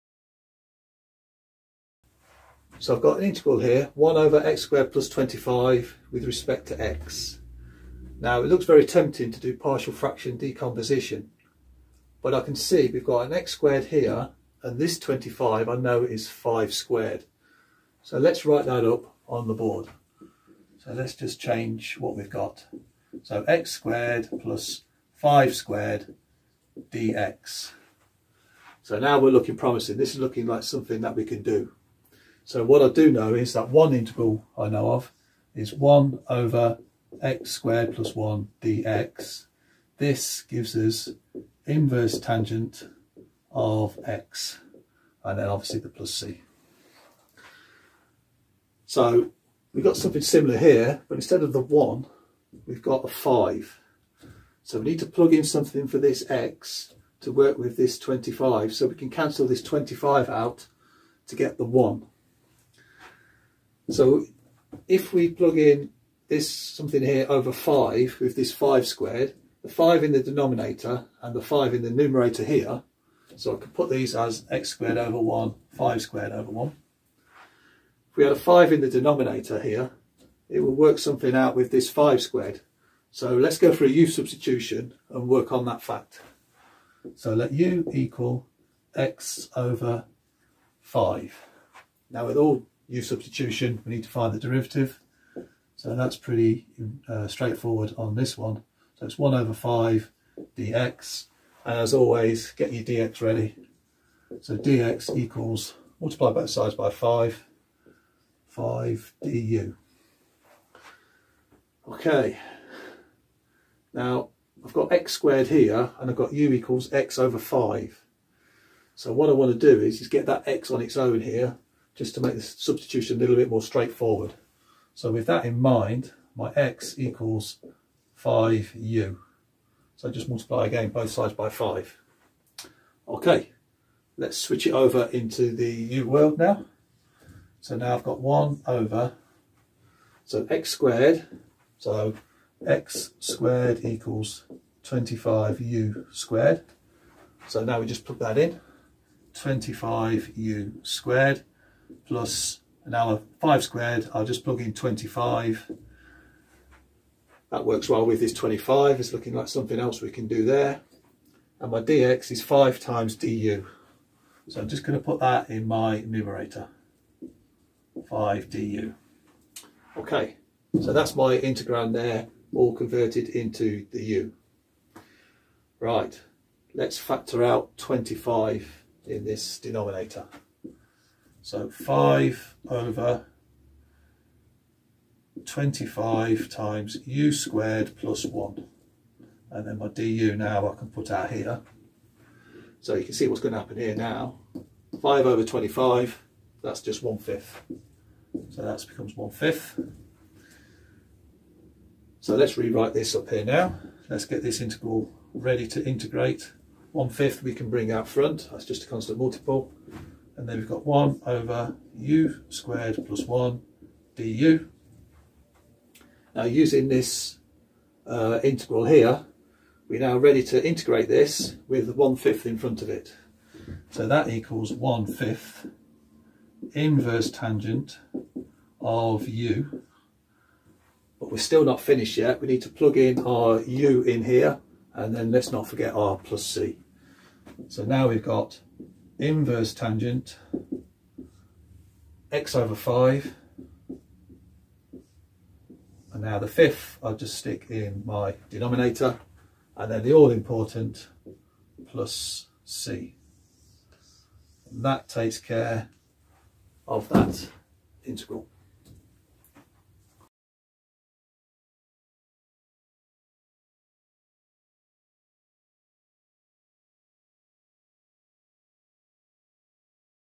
What is the Indefinite Integral for 1/x^2+25 dx
This video shows how to find the Indefinite Integral for 1/x^2+25 dx .
We use a u- substitution for x/5
Then we aim for the inverse tangent function
Then the rest is Algebra